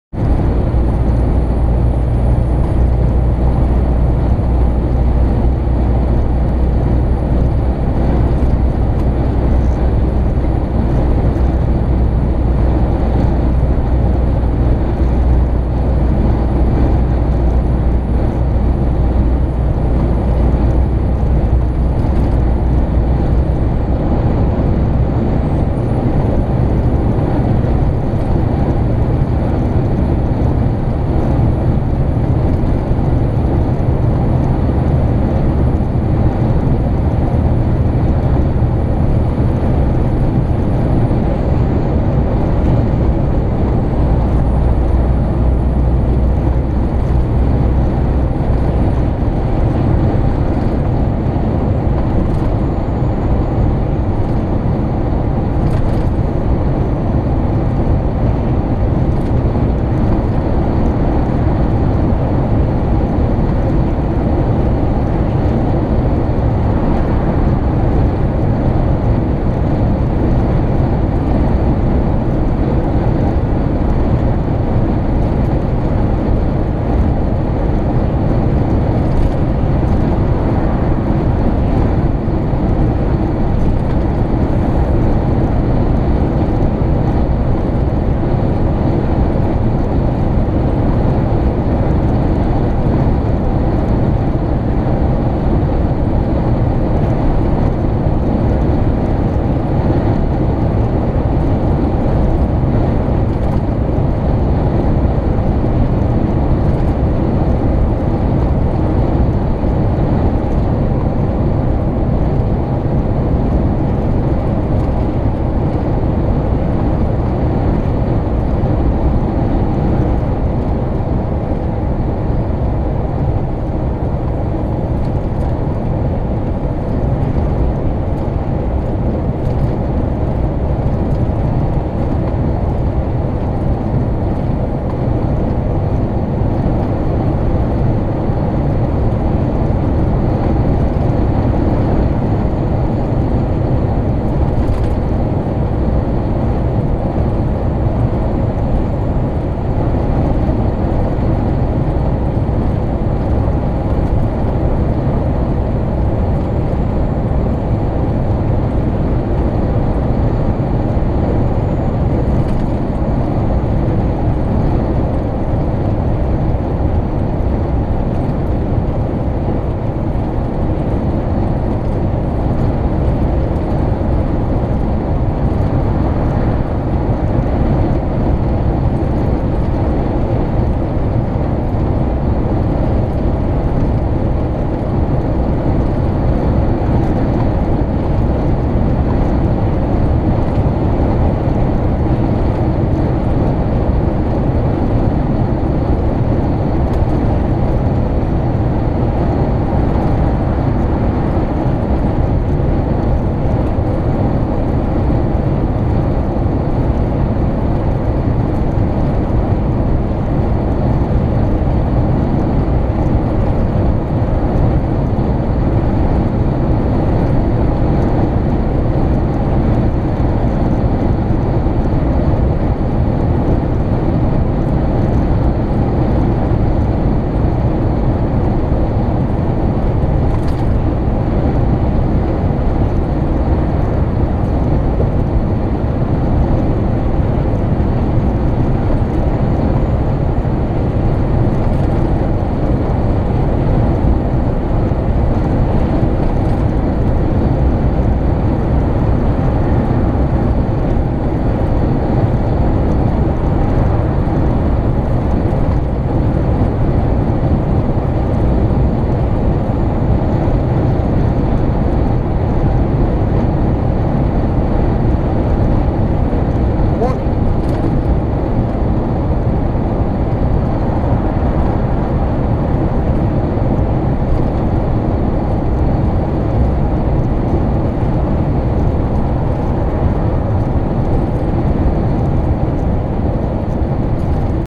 A Drive Through The Cumbrian Hills
This section of the M6 Northbound through Cumbria is one of the most scenic sections of motorway in the U.K.